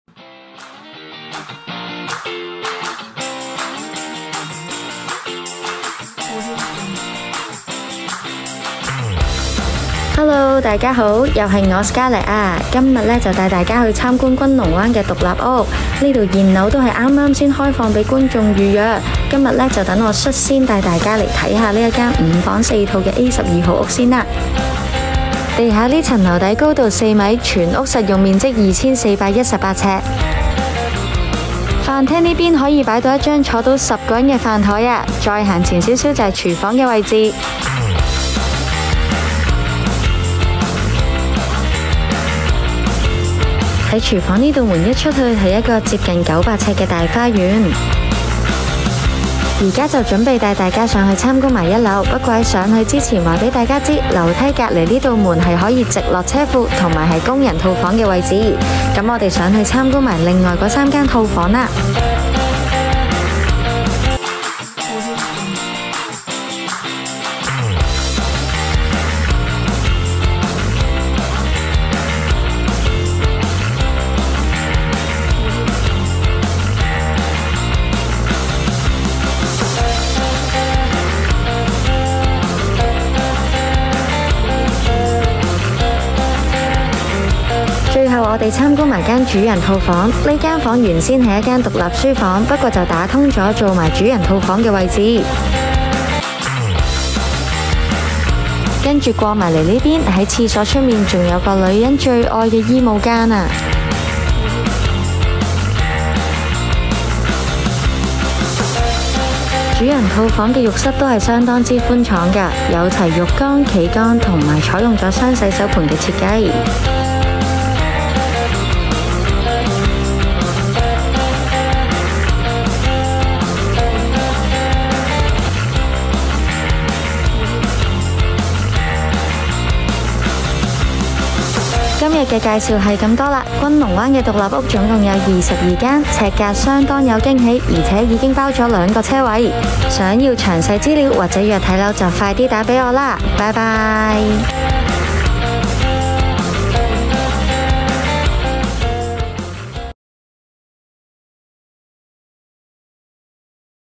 【代理Scarlett推介】Emerald Bay獨立屋
KOL帶睇樓 - Scarlett Sun (S-564516)樓盤推介：

🏠Emerald Bay獨立屋（物業編號：E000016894）

物業地址：屯門 管翠路8號
單位設施/特色：會所、娛樂設施、寵物天地
廣告日期：18/1/2022

屋苑：恆大．珺瓏灣Emerald Bay 30座 1982伙
校網/校區：小學: 71中學: 屯門